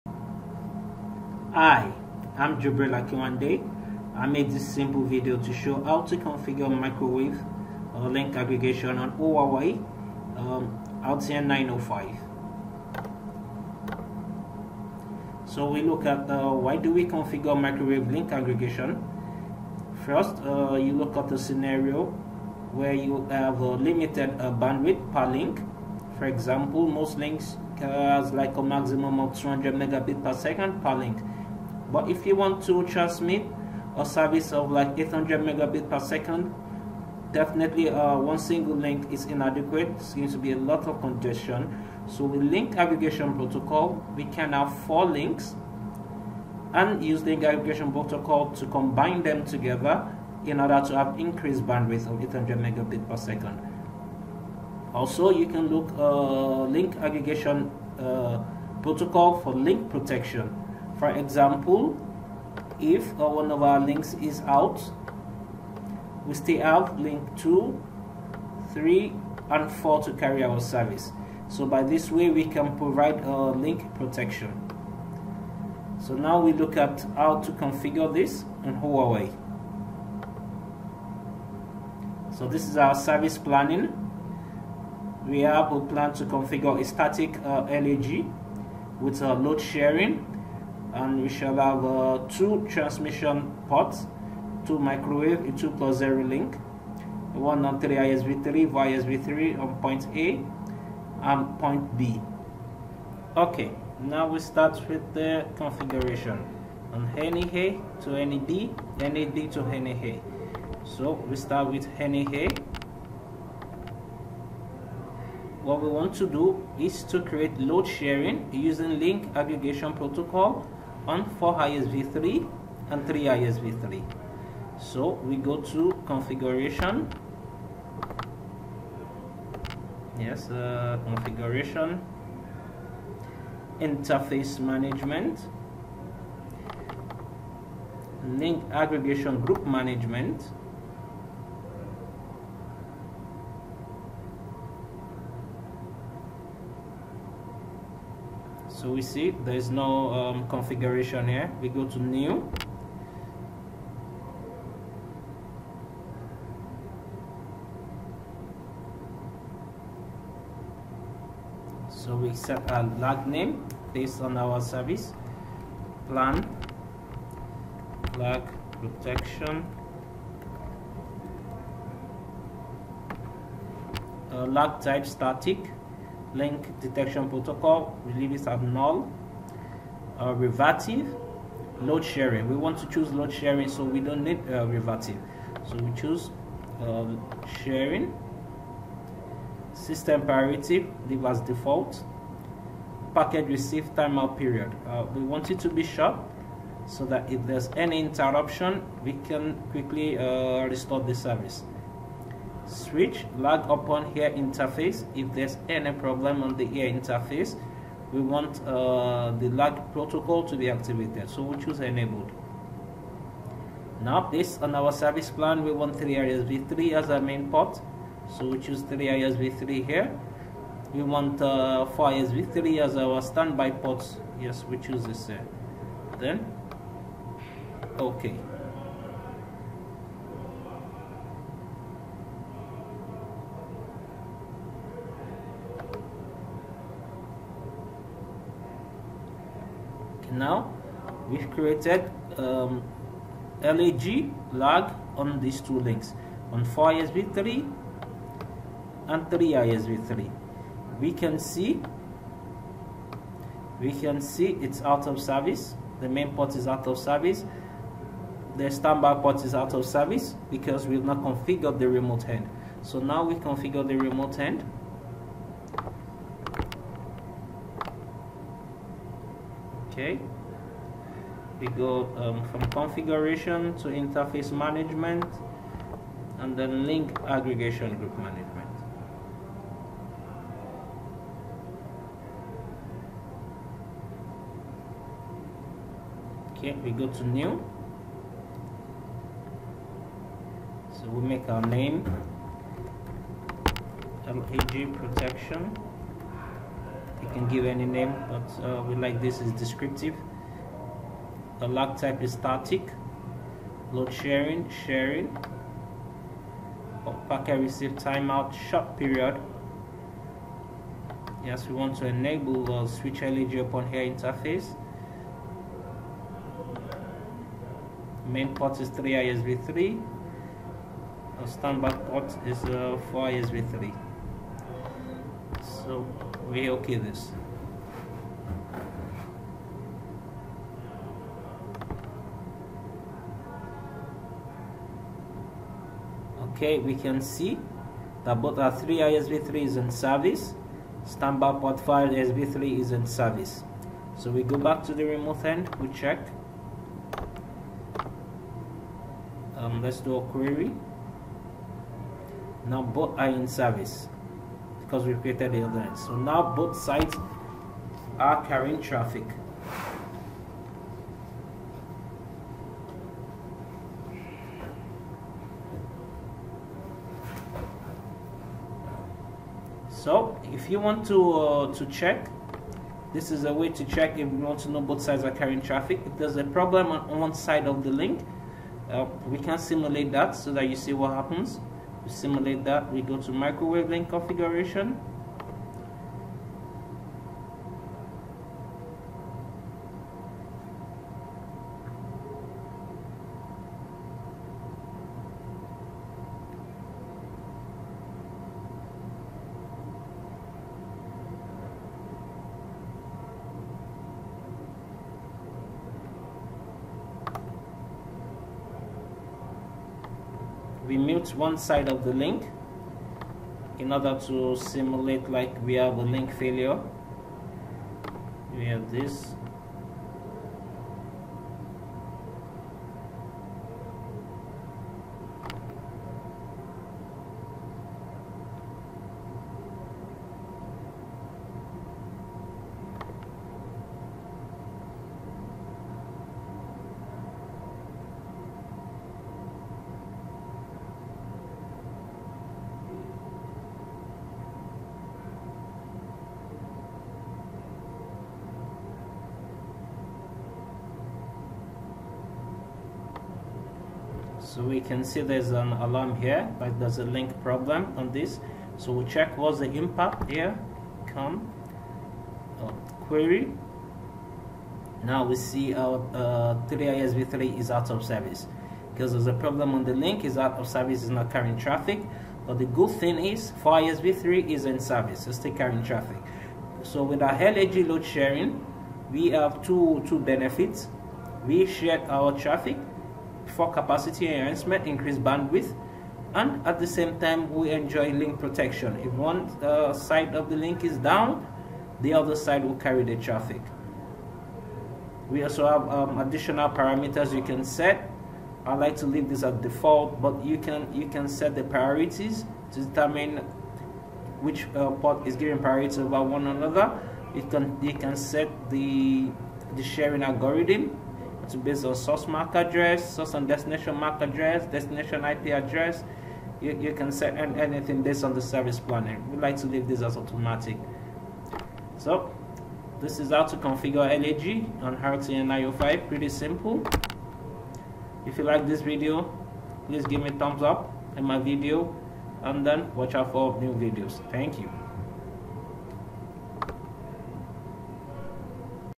How to Configure LAG on Huawei RTN900
A simple video on how to configure Microwave Link Aggregation on Huawei RTN905